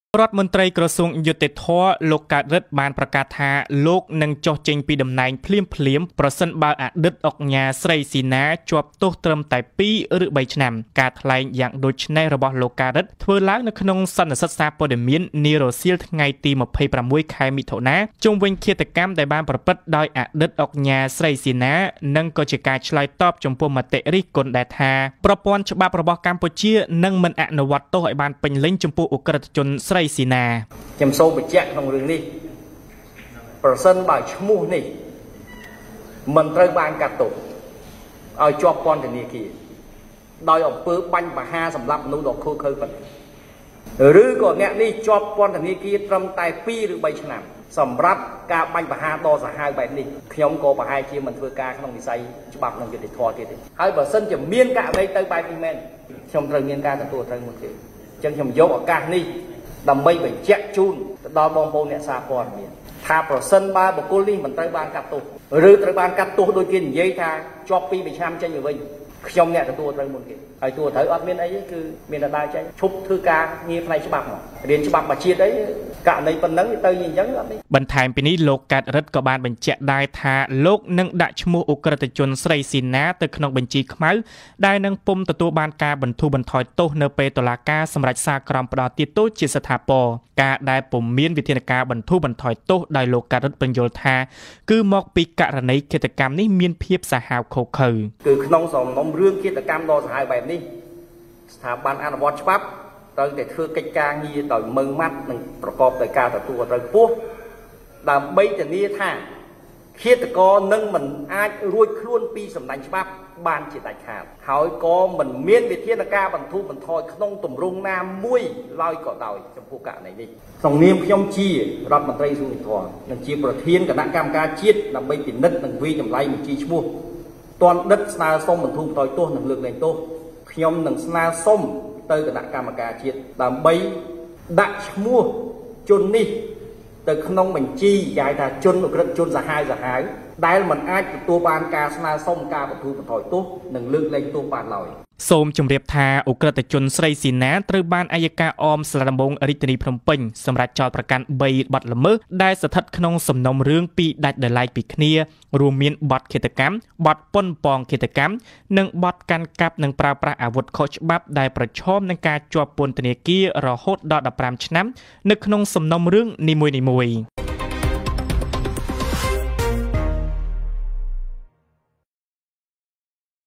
លោកកើត រិទ្ធសុខចិត្តចុះចេញពីតំណែង បើអតីតឧកញ៉ាស្រី ស៊ីណាជាប់ទោសត្រឹម២ ឬ៣ឆ្នាំ
រដ្ឋមន្ត្រីក្រសួងយុត្តិធម៌លោក កើត រិទ្ធបានប្រកាសថា លោកនឹងចុះចេញពីតំណែងភ្លាមៗ ប្រសិនបើអតីតឧកញ៉ាស្រី ស៊ីណាជាប់ទោសត្រឹមតែ២ ឬ៣ឆ្នាំ។ ការថ្លែងយ៉ាងដូច្នេះរបស់លោកកើត រិទ្ធធ្វើឡើងនៅក្នុងសន្និសីទសារព័ត៌មាននារសៀលថ្ងៃទី២៦ ខែមិថុនាជុំវិញឃាតកម្មដែលបានប្រព្រឹត្តិដោយអតីតឧកញ៉ាស្រី ស៊ីណា និងក៏ជាការឆ្លើយតបចំពោះមតិរិះគន់ដែលថា ប្រព័ន្ធច្បាប់របស់កម្ពុជានឹងមិនអនុវត្តទោសឱ្យបានពេញលេញចំពោះឧកិ្រដ្ឋជនស្រី ស៊ីណា។